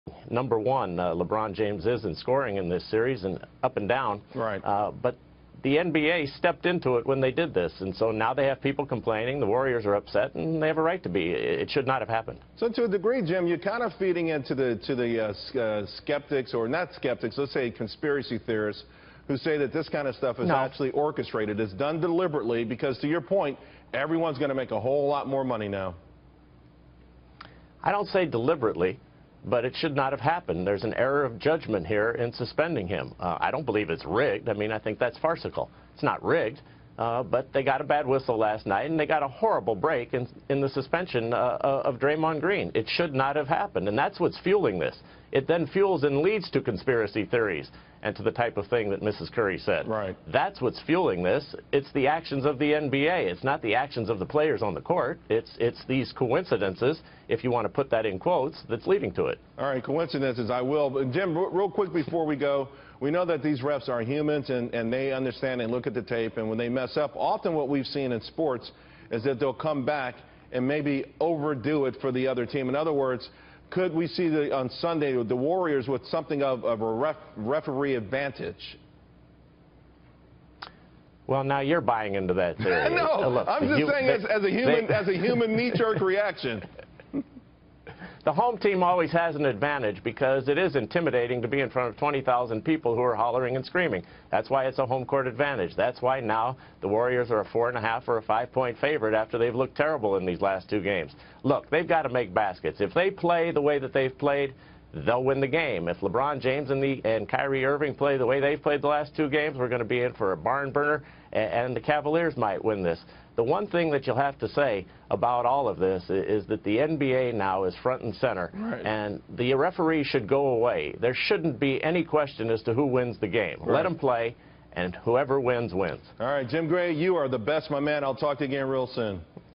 Jim Gray: The NBA is not rigged
Sportscaster Jim Gray discusses Steph Curry’s wife accusing the NBA of being rigged.